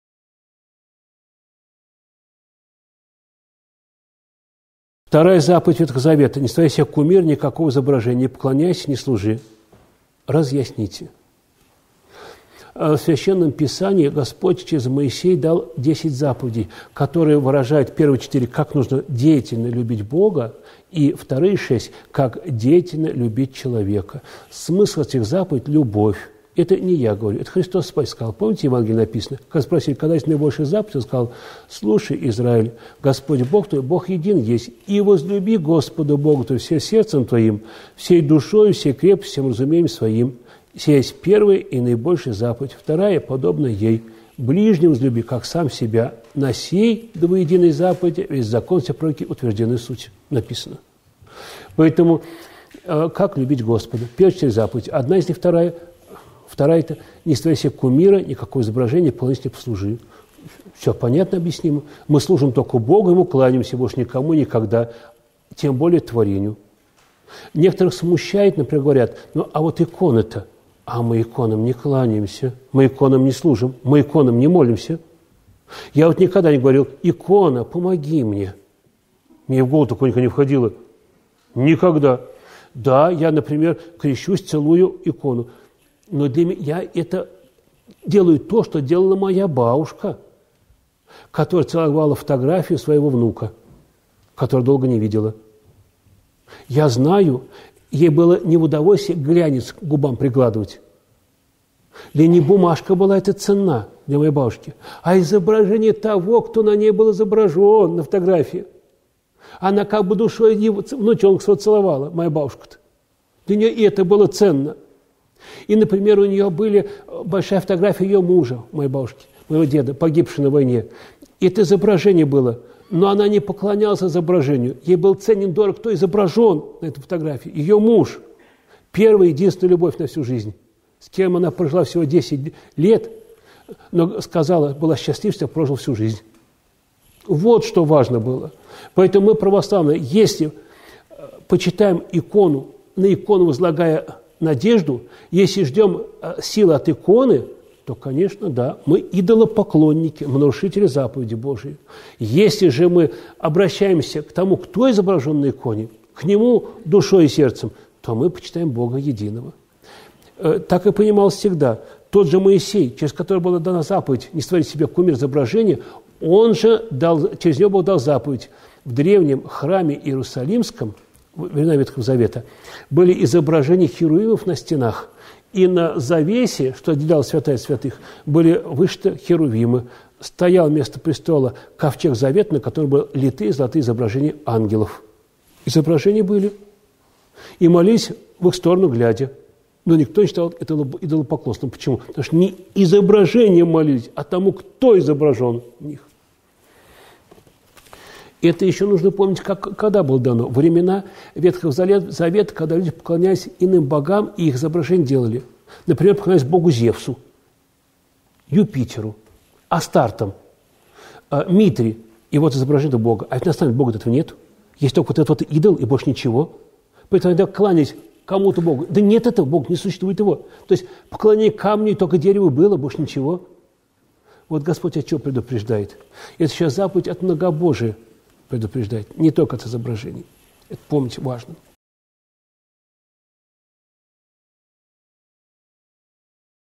О второй заповеди Декалога "Не сотвори себе кумира" (прот. Владимир Головин, г. Болгар)
Фрагмент ответов от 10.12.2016 г.